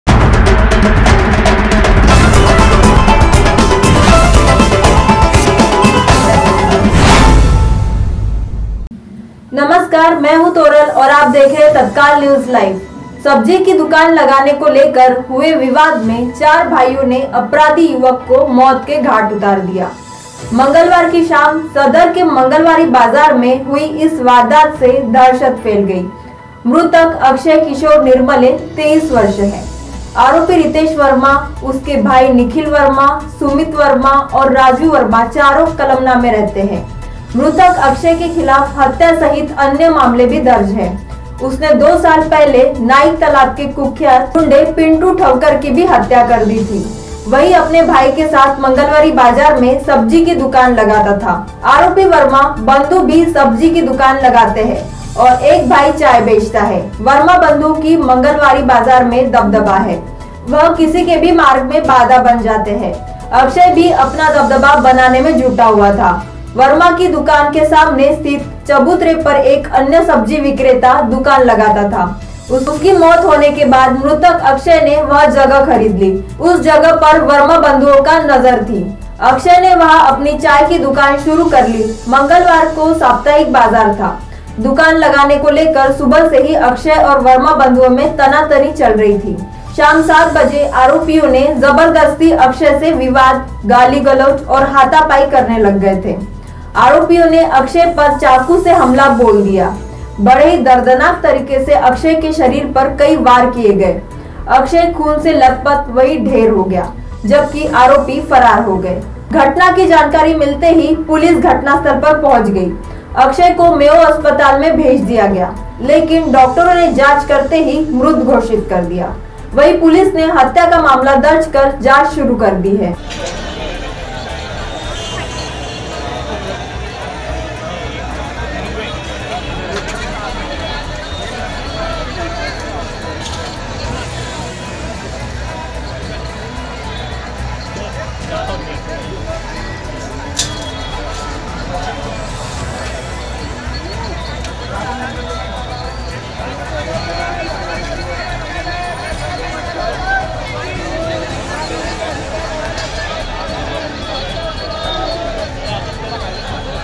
नागपुर सदर मंगलवारी बाजार में सब्जी विक्रेता को उतारा मौत के घाट || Sader Crime News
नागपुर सदर मंगलवारी बाजार में सब्जी विक्रेता को उतारा मौत के घाट || sader crrime news || tatkalnewslive